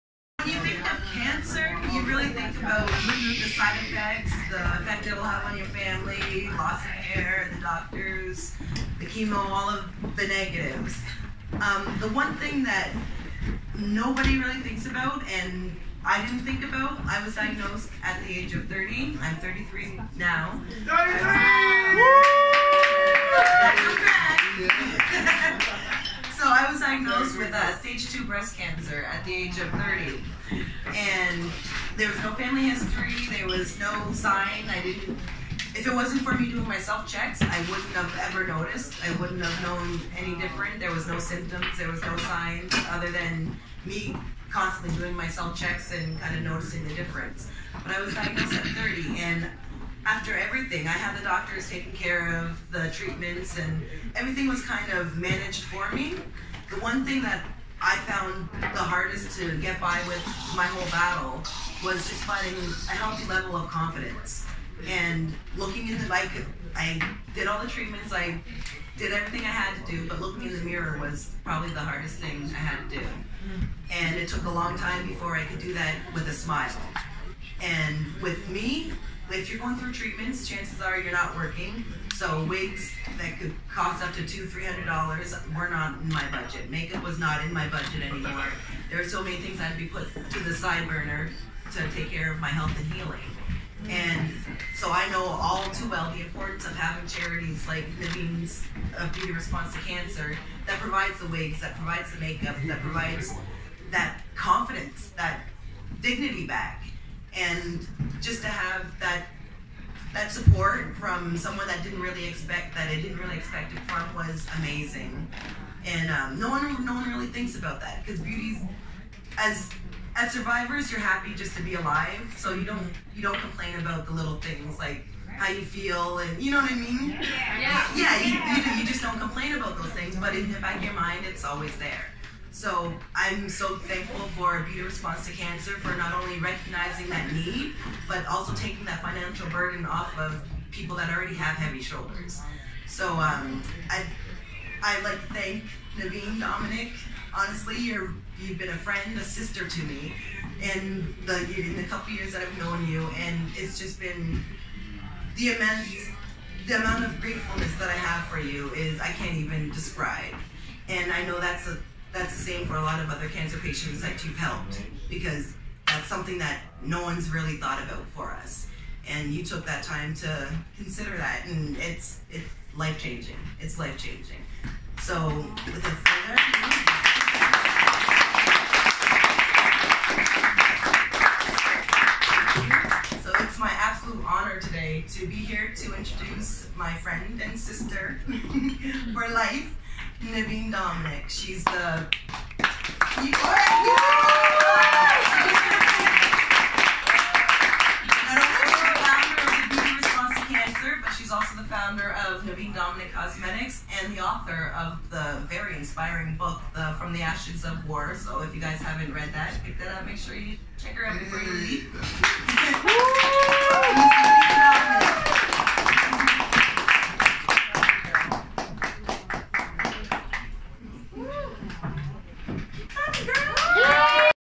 My first public speech in forever
I had the honor to introduce Neveen Dominic at her 4th year anniversary black tie celebration in New York. I was also named an ambassador for her Beauty Response to Cancer Charity. I didn't even shake!!